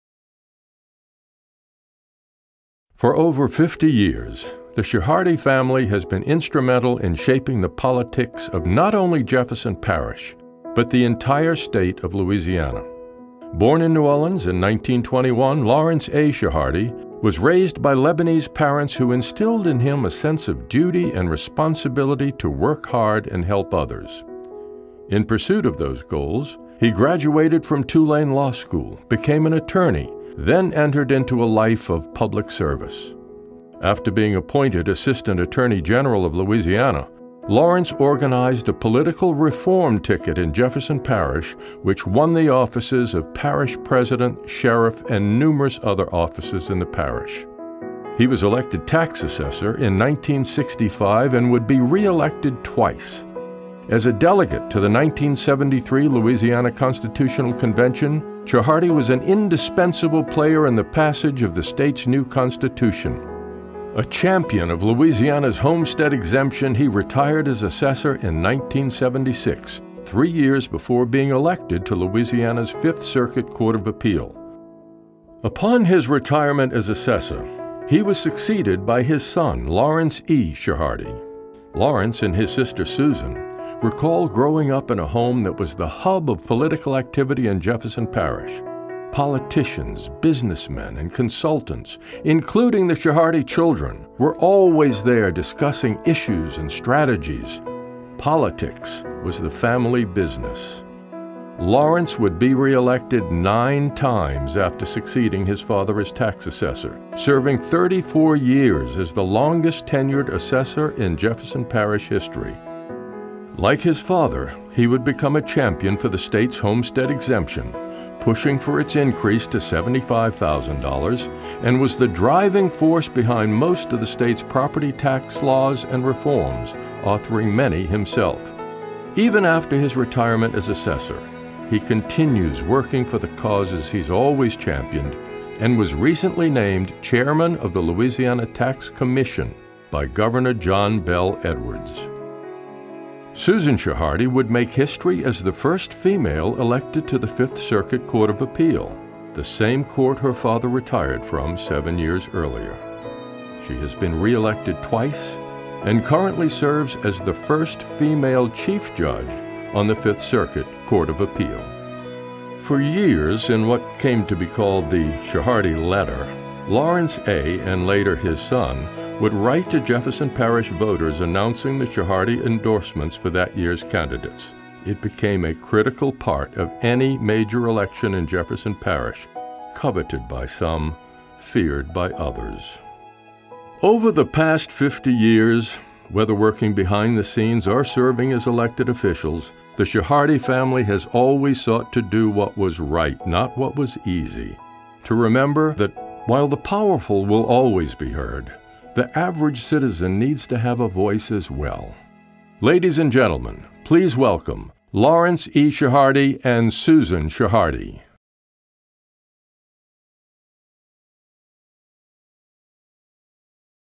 Chehardy Family
The Chehardy Family
Lawrence A. Chehardy: Lawrence was married for 54 years to Louise Ramoneda Chehardy and they had three children, Lawrence E., Carolyn, and Susan. Lawrence A. Chehardy graduated Tulane University School of Law and was admitted to the practice of law in Louisiana. Lawrence was appointed Assessor of Jefferson Parish in November 1965 and in 1966 was elected Assessor of Jefferson Parish to fill out the unexpired term of the late assessor. He was re-elected in 1967 and 1971. Lawrence A. was a Delegate to the Louisiana Constitutional Convention and credited for passage of the state’s new constitution and was also honored by a proclamation from his fellow delegates for his leadership during the convention. Lawrence A. retired as Assessor of Jefferson Parish in December 1976 and served as Chief Deputy Assessor of Jefferson Parish from 1977 – September 1979.After briefly exploring a run for Governor in 1979, Chehardy was elected Judge of the Fourth Circuit Court of Appeal in 1979 and was re-elected and served until March 1991 after reaching the mandatory judicial retirement age of 70.
Lawrence E. Chehardy: In 1977 Lawrence E. Chehardy assumed the office of Tax Assessor of Jefferson Parish.  During his 34 years of service he was an integral part of Jefferson Parish’s as well as the state’s political landscape successively supporting candidates for various offices.  His track record of political successes is quite long.  He is most noted for his success in defending Louisiana’s homestead exemption.  His popularity with the public in Jefferson Parish as well as around the metropolitan area and state is noteworthy.  In many cases his support for the underdog candidate resulted in a win against a better know and better financed candidate.  During his tenure the Republican Party saw tremendous growth.  In 1995 Lawrence Chehardy switched from democrat to republican.  During his tenure the Jefferson Parish Assessor’s Office became a model of efficiency and progressive practices through the use of computers and software designed by his office.
Susan M. Chehardy: Susan received her Juris Doctorate from Loyola University of the South Law School in 1985. In August of 1986 she was employed as an associate with the firm of Gauthier, Murphy, Chehardy, Sherman & Breslin, engaged in the practice of personal injury litigation. In 1989 she became a partner in the firm of Chehardy, Sherman, Ellis & Breslin. In 1991, she opened her own general litigation practice, Chehardy & Nielsen. In 1992 Susan was elected judge of Division N of the 24th JDC. She served as judge of Division N, winning re-election without opposition to her district court seat in 1996. In  1998 she was elected to Division D of the Fifth Circuit Court of Appeal. In Jan. 2013, Susan was sworn in to her new term as the first female Chief Judge of the Louisiana Fifth Circuit Court of Appeal.